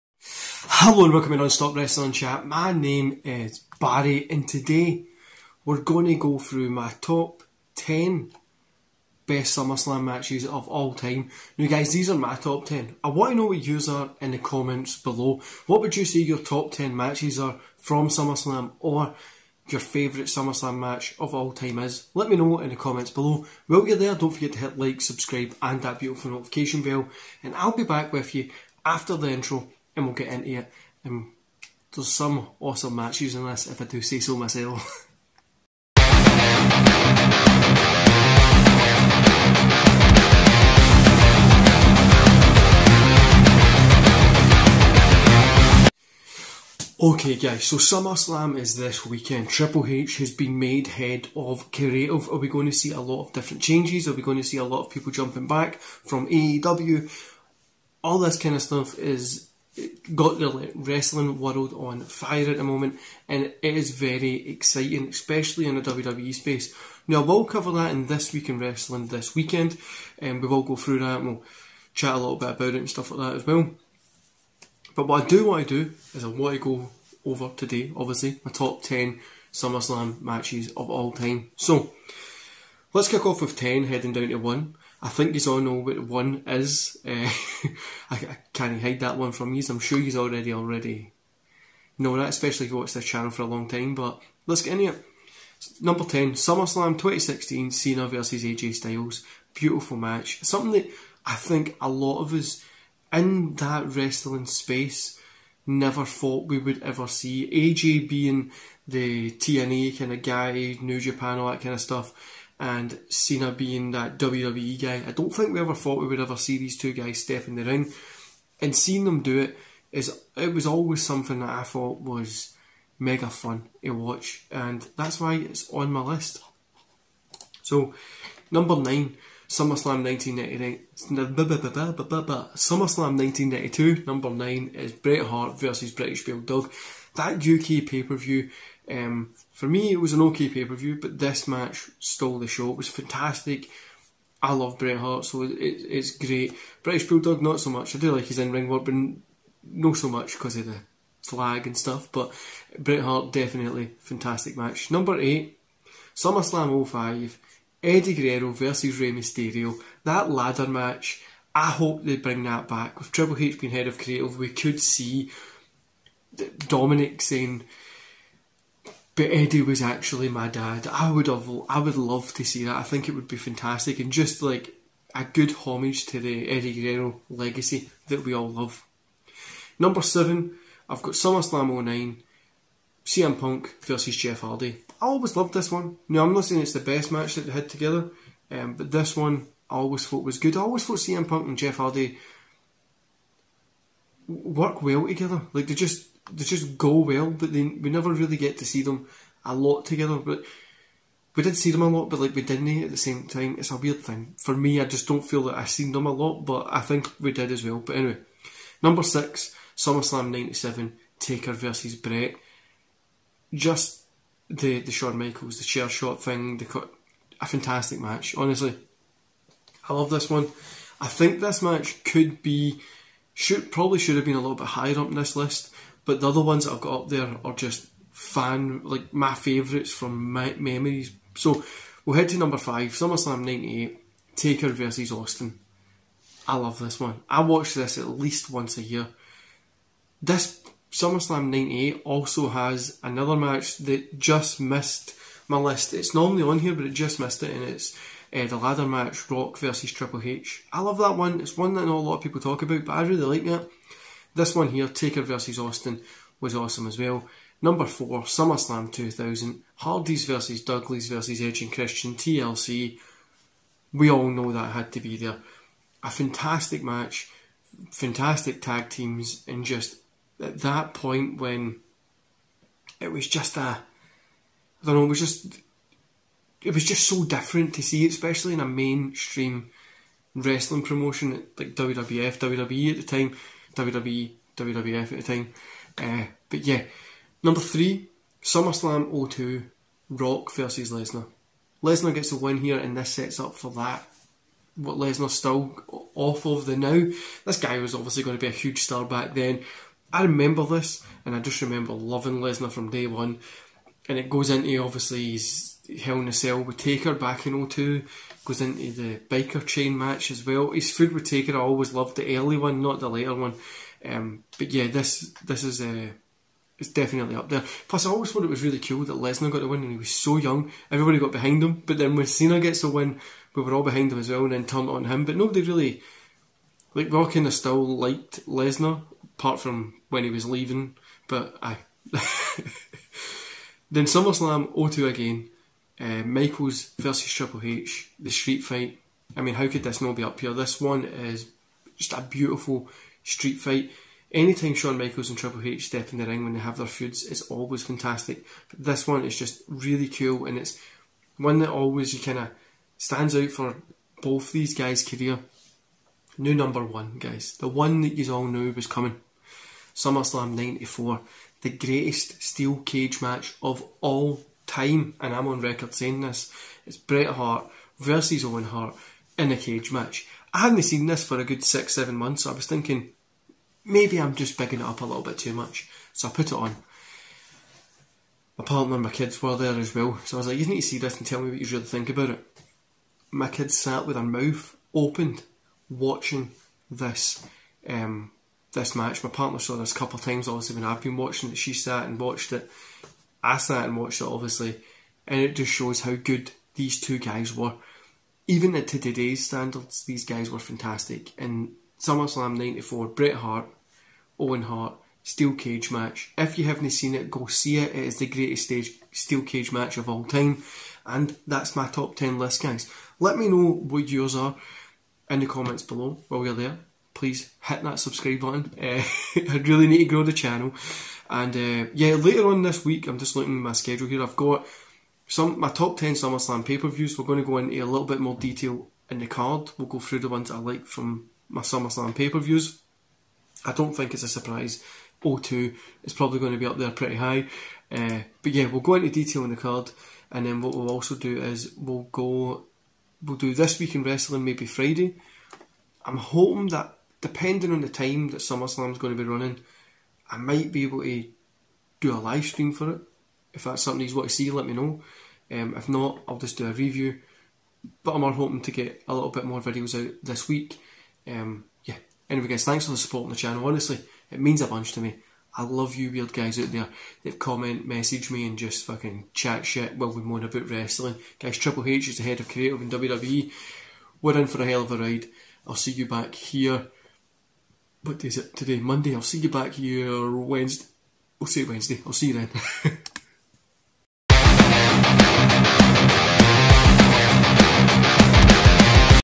Top 10 SummerSlam Matches of All Time
So let's talk about my top 10 SummerSlam matches of all time. Don't forget to let me know your ones in the comments below.